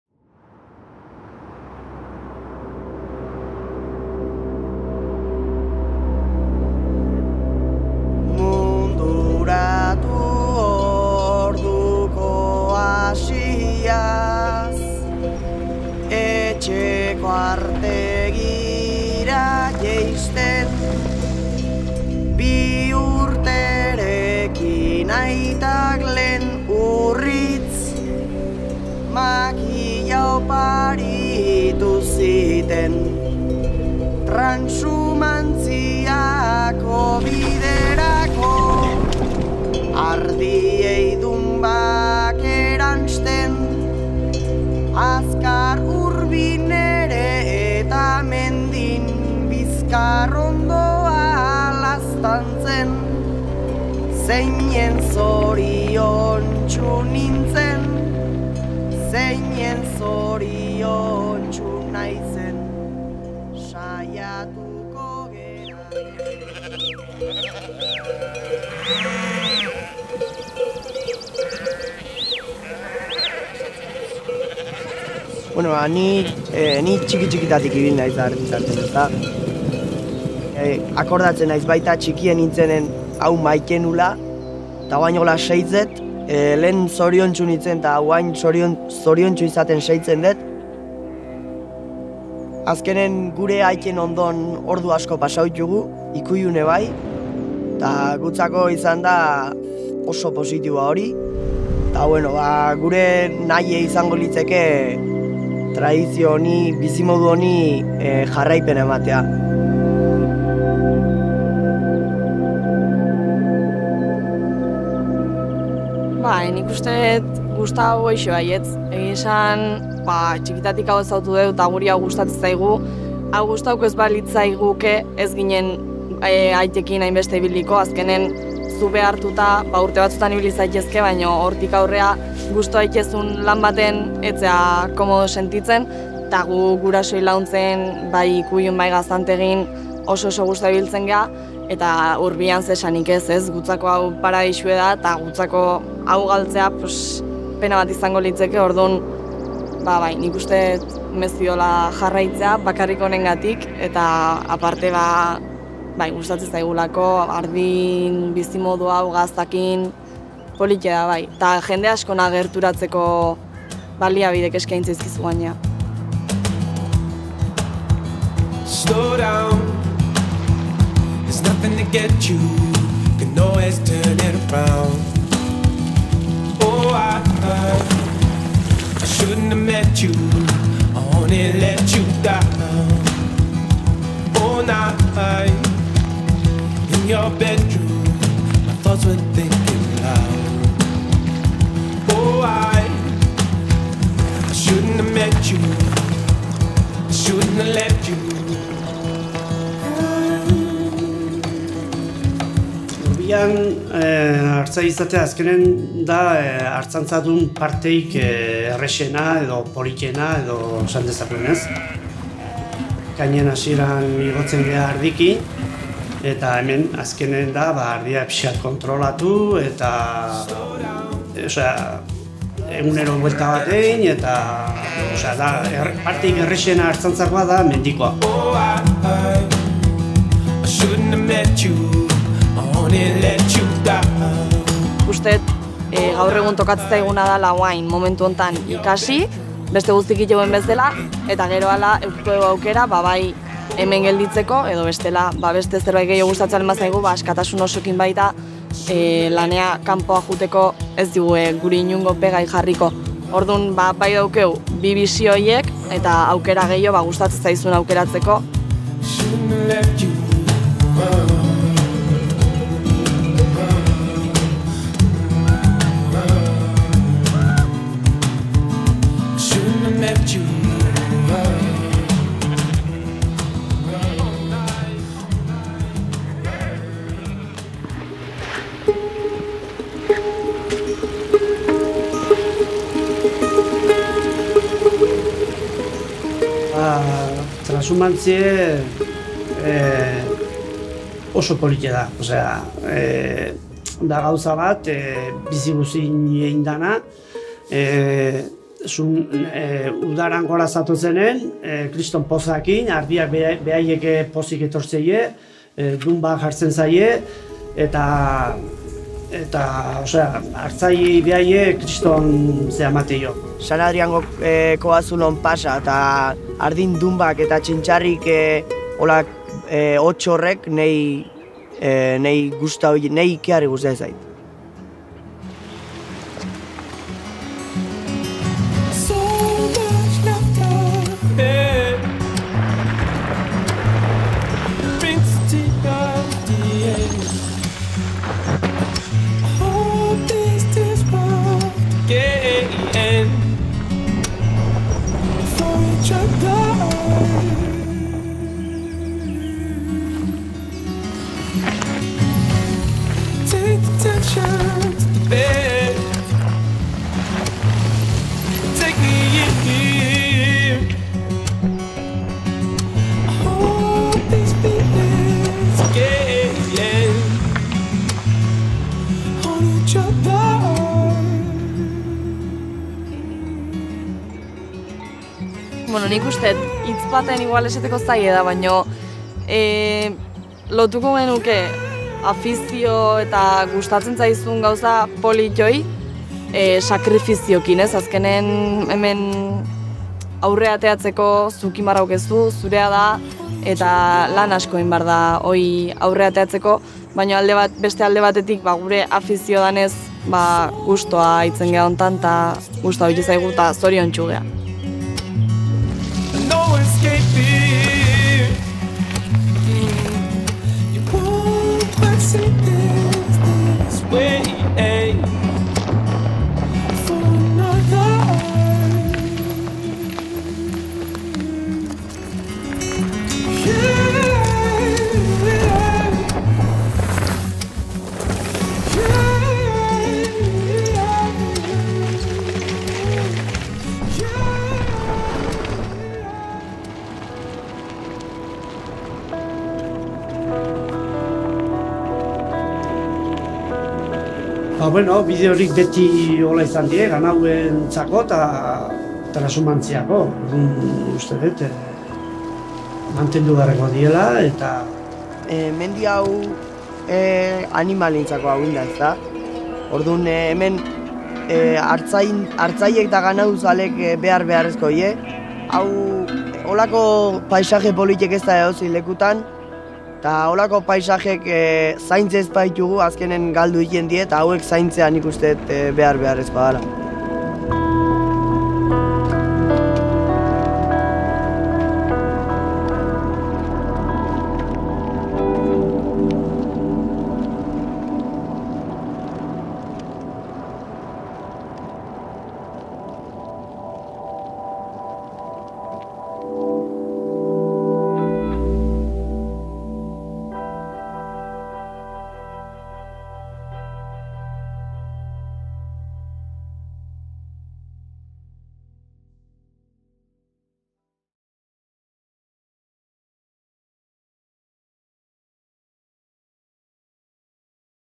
Shepherds of the Trashumancia | TRAIL TALES
Artzain transhumanteak.

Antzinako tradizio eta bide naturalek gaur egungo mendi-bideak utzi dizkigute. Mendeak dira sortu zirenetik. Zibilizazioen igarotzeak, edo urtaro bakoitzean haran artean ganadua garraiatzeak, gure bizikletekin pedalei eragiteko jardunaldi horiek gogorarazten dizkigute.